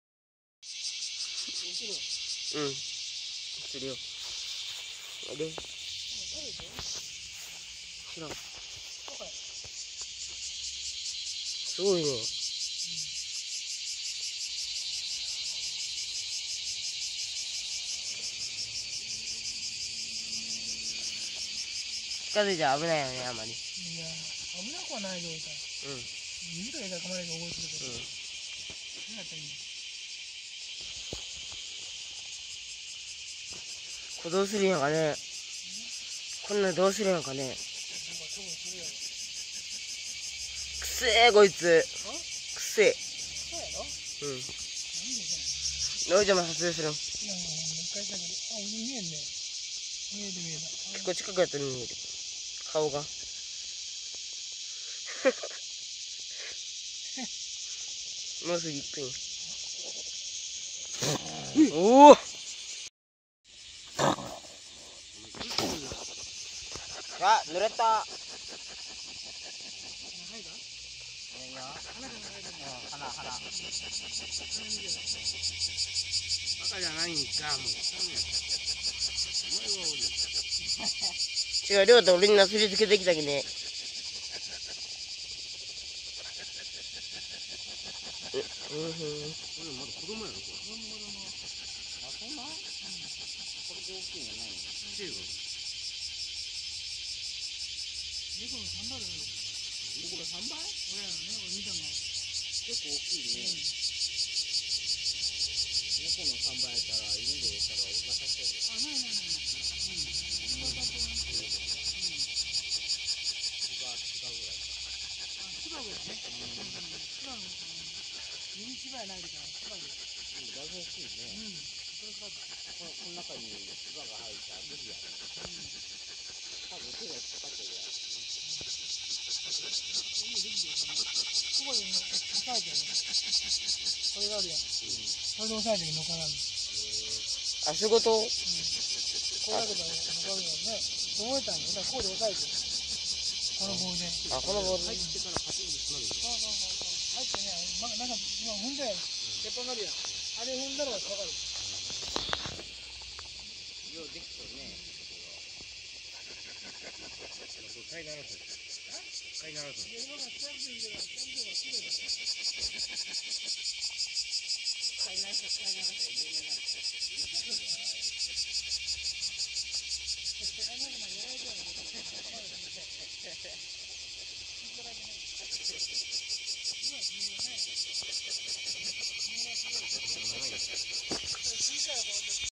アナグマの様子
7/27火曜日に親戚のおいちゃんの畑の罠にかかった時の様子

久しぶりの動画投稿に成りました。

絶対宜しくお願いします。